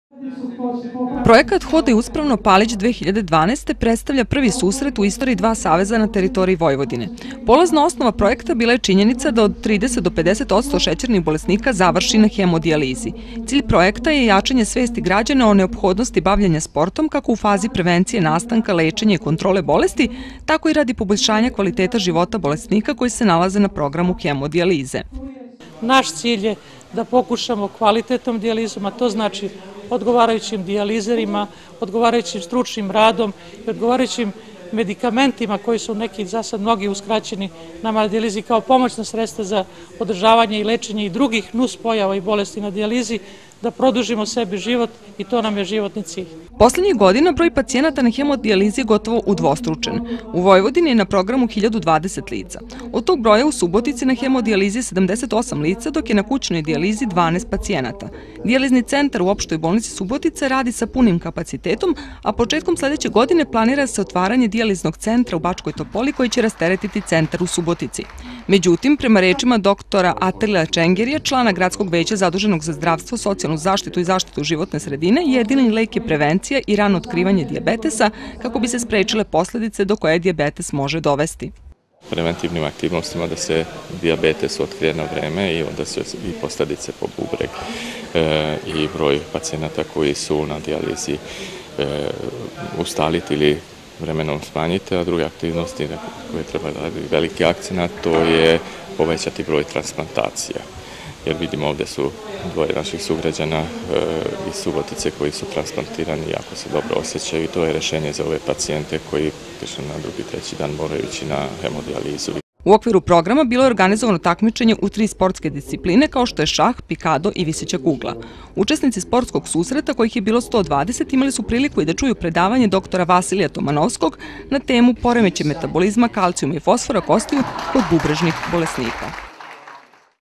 "Hodaj uspravno"
Na Paliću je juče održano prvo sportsko nadmetanje Saveza bubrežnih invalida Vojvodine i Saveza društva za borbu protiv šećerne bolesti pod nazivom „Hodaj uspravno-Palić 2012." U okviru programa, koji je pre svega imao edukativni karakter, organizovano je takmičenje u tri discipline kao što je šah, pikado i viseća kugla, kao i okrugli sto na temu „Rehabilitacija invalidnih lica". Projekat je odobren od strane Ministarstva omladine i sporta.

Projekat „Hodaj uspravno-Palić 2012." predstavlja prvi susret u istoriji dva saveza na teritoriji Vojvodine. Polazna osnova projekta bila je činjenica da od 30 do 50 odsto šećernih bolesnika završi na hemodijalizi. Cilj projekta je jačanje svesti građana o neophodnosti bavljenja sportom, kako u fazi prevencije nastanka, lečenja i kontrole bolesti, tako i radi  poboljšanja kvaliteta života bolesnika koji se nalaze na programu hemodijalize.

Poslednjih godina broj pacijenata na hemodijalizi je gotovo udvostručen. U Vojvodini  je na programu  1020 lica. Od tog broja  u Subotici na hemodijalizi je 78 lica, dok je na kućnoj dijalizi 12 pacijenata. Dijalizni centar u Opštoj bolnici Subotica radi sa punim kapacitetom, a početkom sledeće godine planira se otvaranje dijaliznog centra u Bačkoj Topoli, koji će rasteretiti centar u Subotici. Međutim, prema rečima dr Atile Čengerija, člana GV zaduženog za zdravstvo, socijalnu zaštitu i zaštitu životne sredine, jedini lek je prevencija i rano otkrivanje dijabetesa, kako bi se sprečile posledice do kojih dijabetes može dovesti.

U okviru programa bilo je organizovano takmičenje u tri sportske discipline  kao što je šah, pikado i viseća kugla.  Učesnici sportskog susreta, kojih je bilo 120,  imali su priliku i da čuju predavanje dr Vasilija Tomanoskog na temu „ Poremećaj metabolizma kalcijuma i fosfora kostiju kod bubrežnih bolesnika".